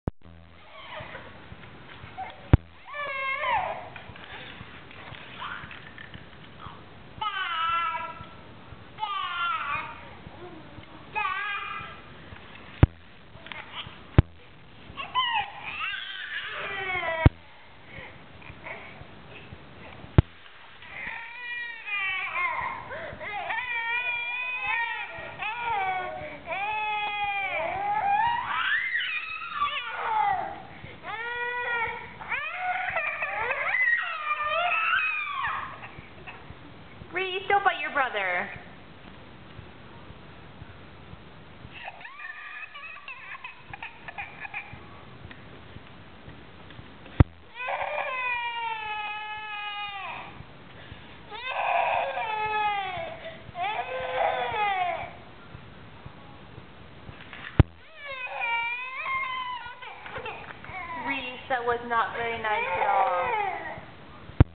dog collar fight - aug 2011
The twins are almost a year old now. They have a house filled with many toys. But they both wanted the dog collar. Which obviously resulted in a fight.